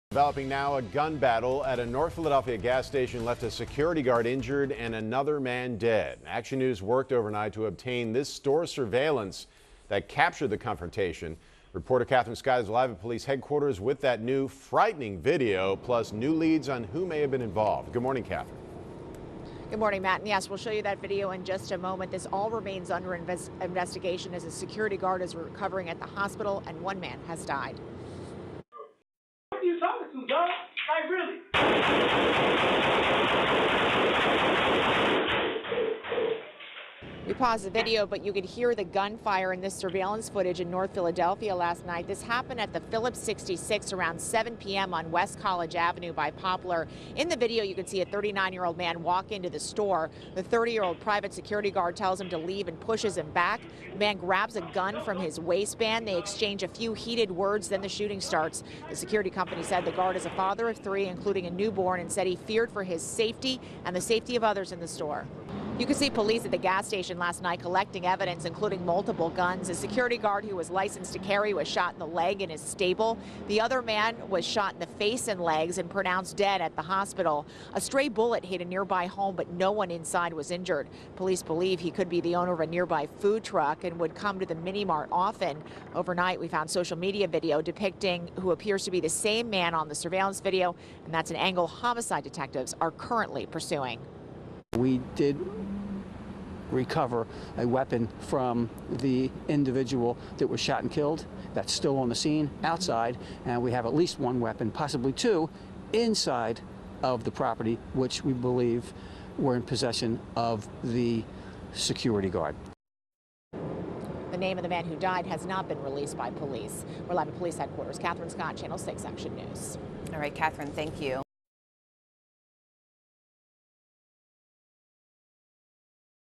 Video shows moments leading to deadly gun battle at gas station
Police say the confrontation involved an armed 39-year-old man and a 30-year-old security guard inside the store.